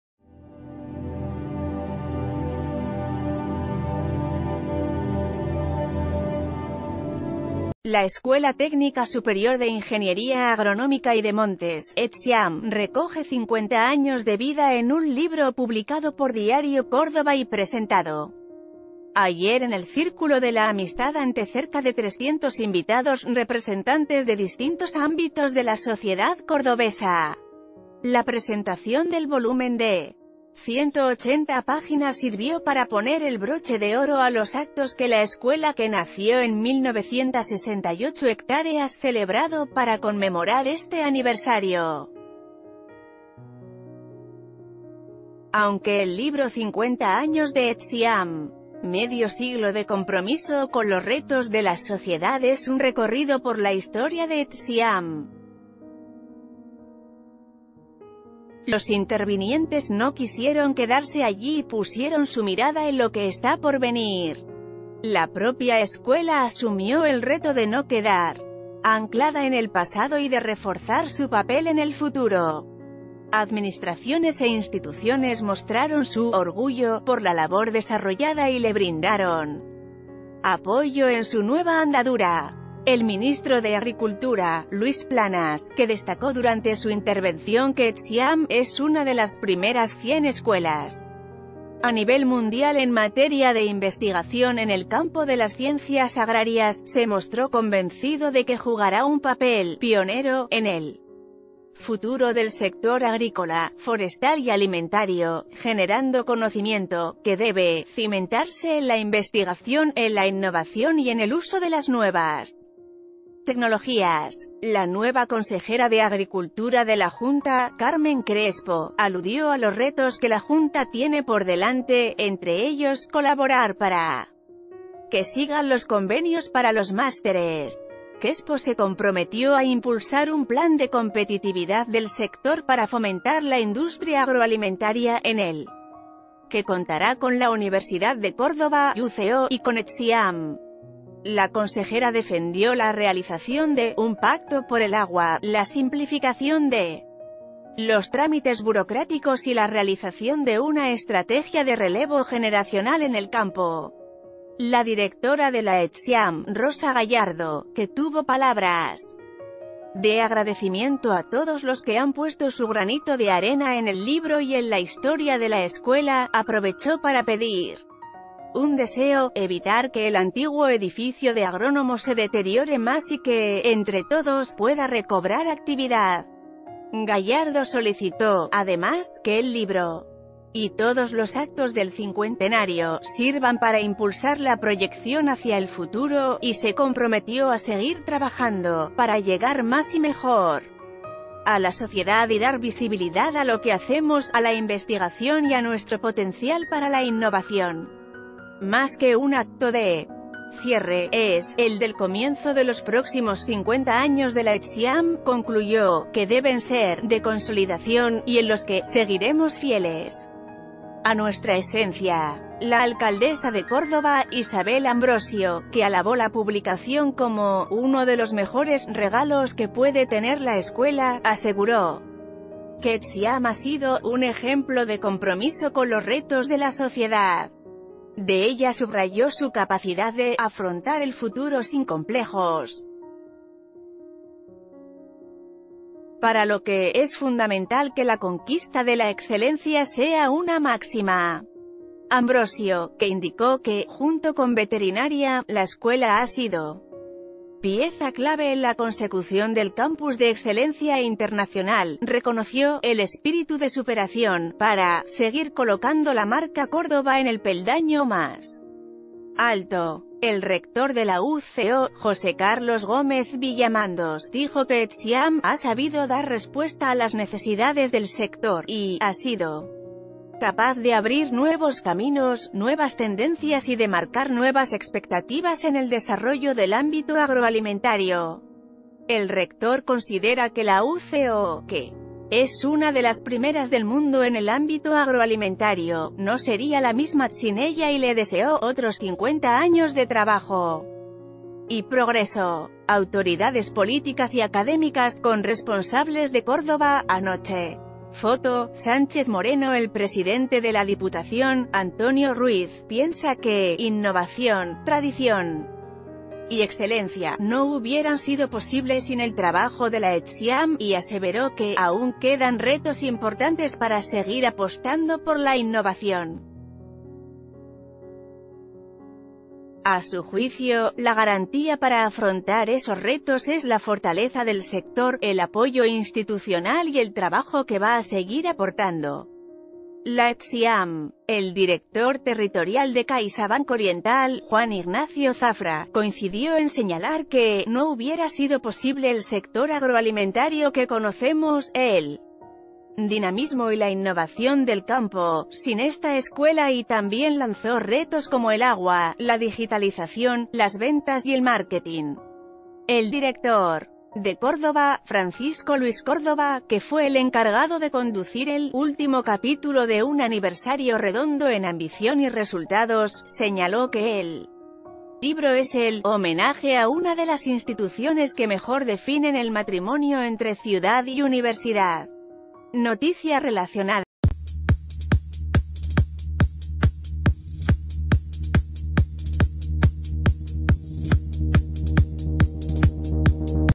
Etsiam recoge 50 a�os de vida en un libro publicado por Diario …
La Escuela Técnica Superior de Ingeniería Agronómica y de Montes (Etsiam) recoge 50 años de vida en un libro publicado por Diario CÓRDOBA y presentado ayer en el Círculo de la Amistad ante cerca de 300 invitados representantes de distintos ámbitos de la sociedad cordobesa. La presentación del volume...